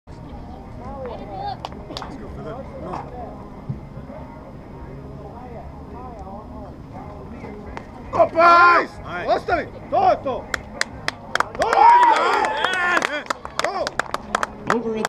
Filip Mihaljević 59.45m (195-0)
Filip Mihaljević
Men Discus 59.45m (195-0)
Virginia Cup 3/21/2015